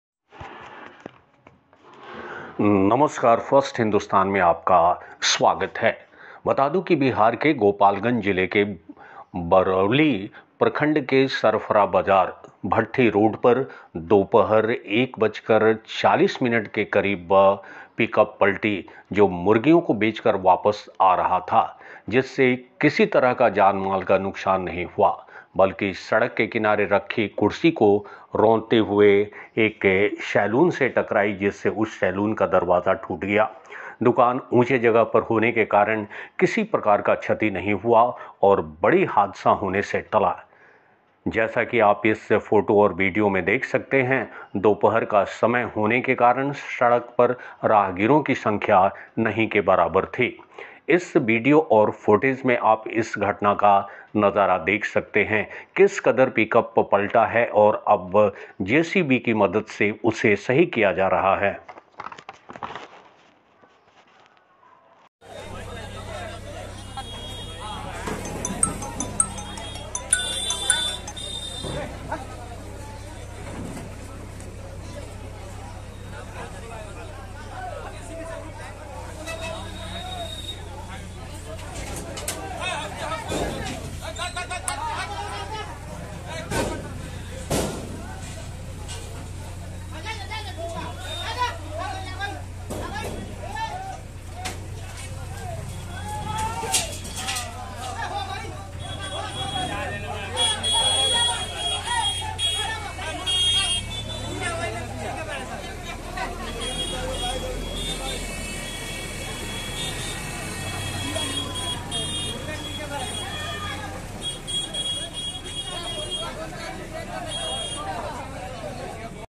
सरफरा बाजार में पिकअप पलटी , सैलून से जाकर टकराई सलून में ग्राहक भी मौजूद थे /First Hindustan
सरफरा बाजार में पिकअप पलटी , सैलून से जाकर टकराई सलून में ग्राहक भी मौजूद थे /